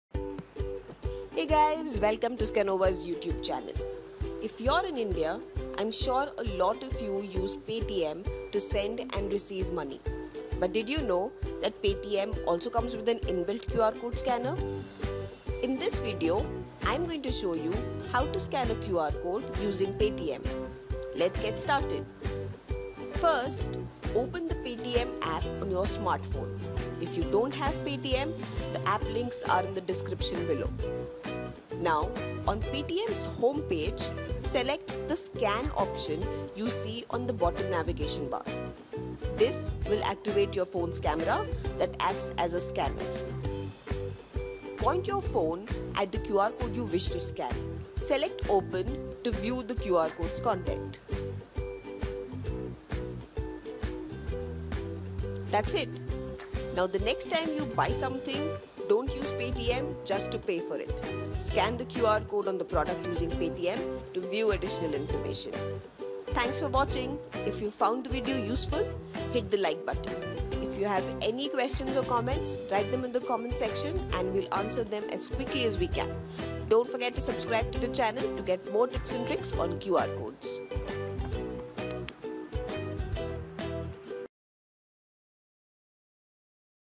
How to scan any QR Code using Paytm
Hey everyone,

In this video, you will see how you can scan a QR Code using Paytm without having to install any other QR Code scanning app on your phone.

About the video:
Did you know that Paytm comes with an inbuilt QR Code scanner? In this video, learn how to scan a QR Code using Paytm.

Other Links:
Paytm QR Code Scanner Allows Users to Scan Any QR Code

If you have any queries or concerns, write them in the comments below.